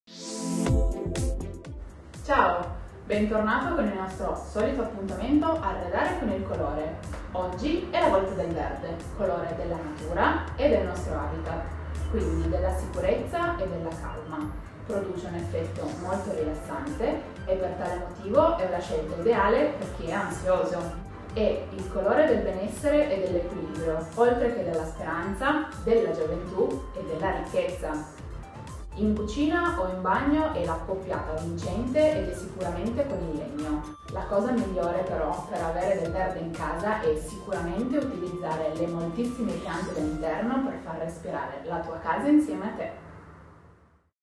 IL COLORE DELLA NATURA IL VERDE!
Ed Ecco il colore Verde 🟢🟩 colore della natura 🌳 e del nostro habitat, quindi della
sicurezza e della calma, produce un effetto rilassante📗 per tale motivo è
la scelta ideale per chi è ansioso.

E' il colore del benessere e dell'equilibrio oltre che della Speranza, della gioventù
 e della ricchezza!🤑

In cucina o in bagno l'accoppiata vincente è sicuramente con il legno 🎋.

La cosa migliore però per avere del verde in casa è sicuramente utilizzare
le moltissime piante 🌵🌿🪴🌾 da interno per far respirare la tua casa insieme a te!